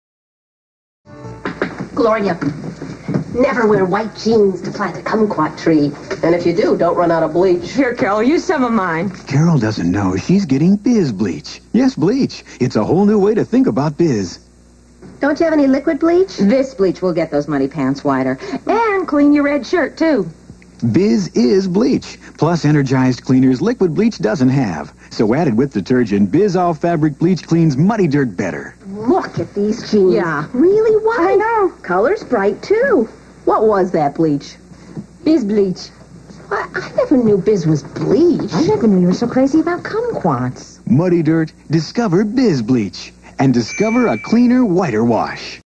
1982 Biz All-Fabric Bleach "White jeans, planting Kumquat trees" TV Commercial
This commercial aired on January 10th, 1983. It was shown during an airing of the movie "Key Largo" on Channel 5 KTLA. KTLA was an independent station at that time.